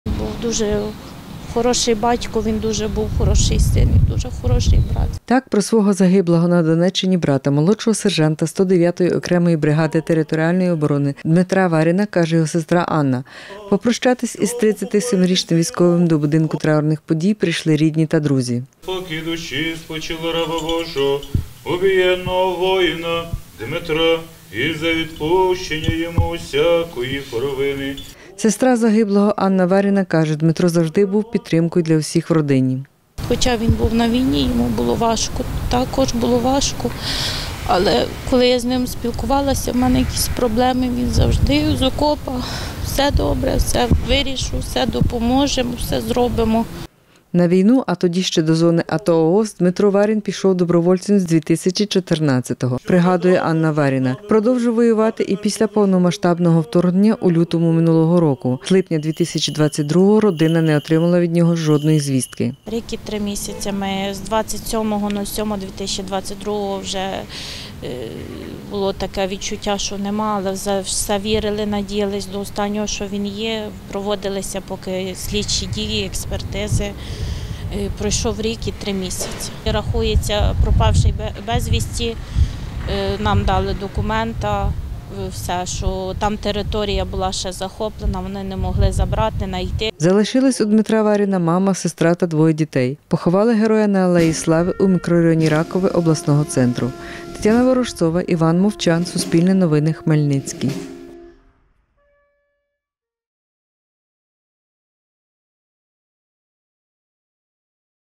У Хмельницькому попрощалися із полеглим військовослужбовцем Дмитром Варіним
У Хмельницькому попрощались із військовослужбовцем. 37-річний Дмитро Варін загинув у Волноваському районі на Донеччині. Чоловік був молодшим сержантом 109-ої окремої бригади територіальної оборони.